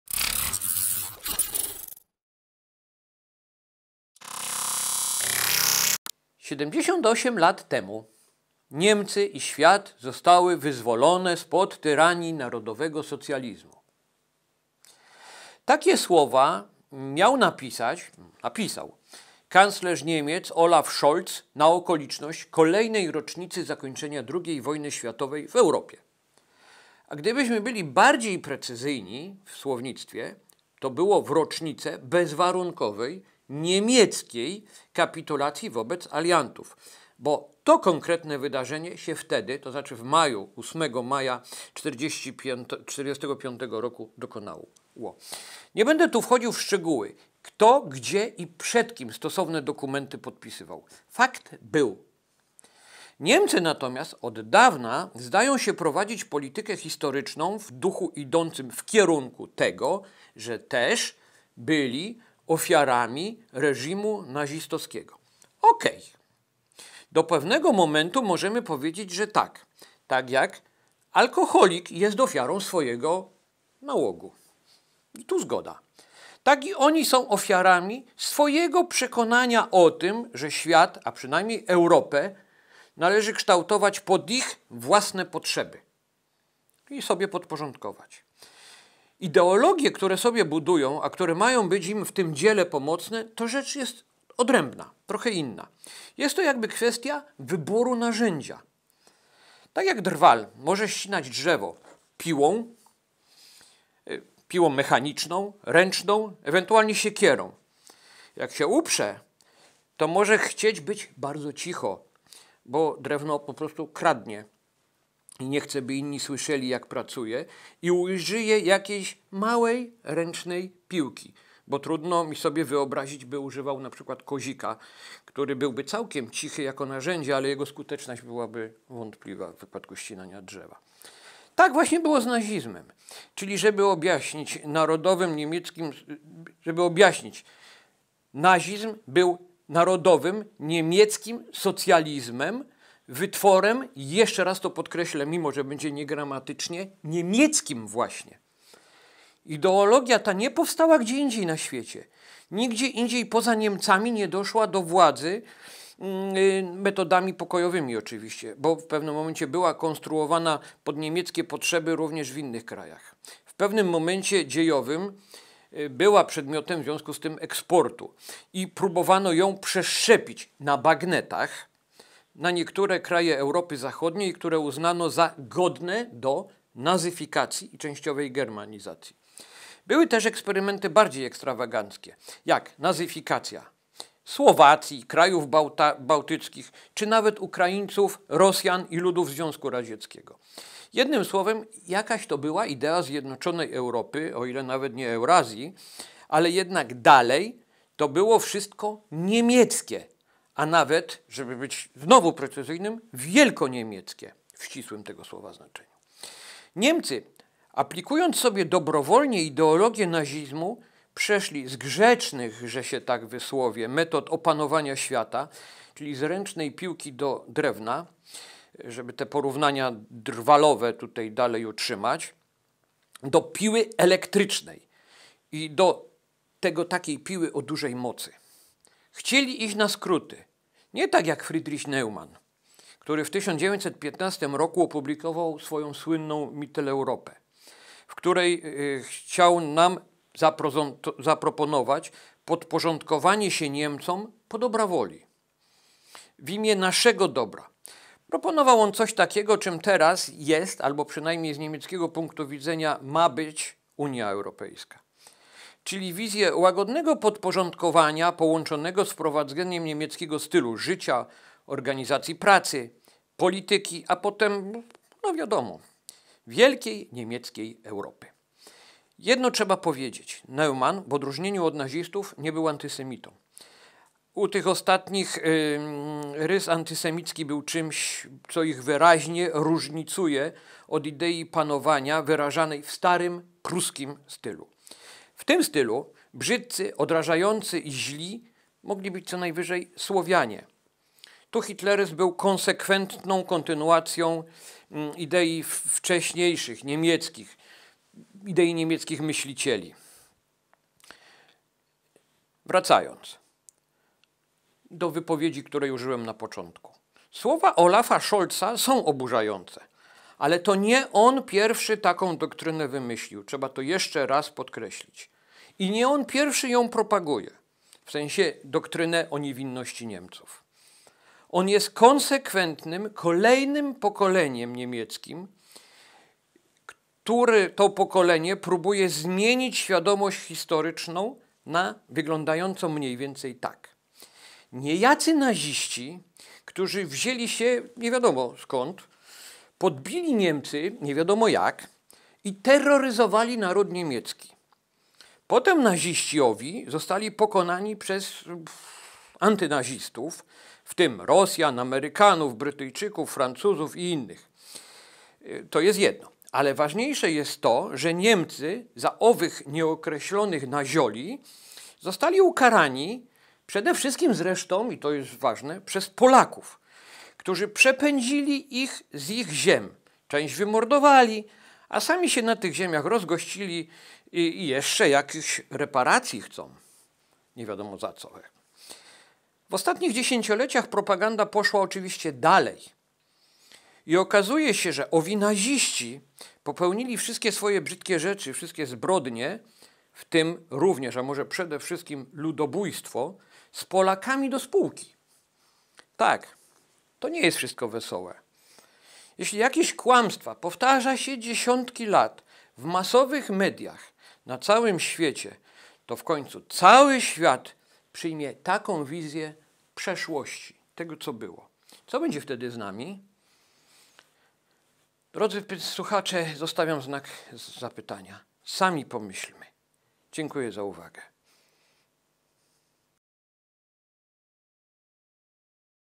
(Za)krótko: Nazizm miał narodowość!
"78 lat temu Niemcy i świat zostały wyzwolone spod tyranii narodowego socjalizmu". Takie słowa miał napisać kanclerz Niemiec, Olaf Scholz na okoliczność kolejnej rocznicy zakończenia II wojny światowej w Europie.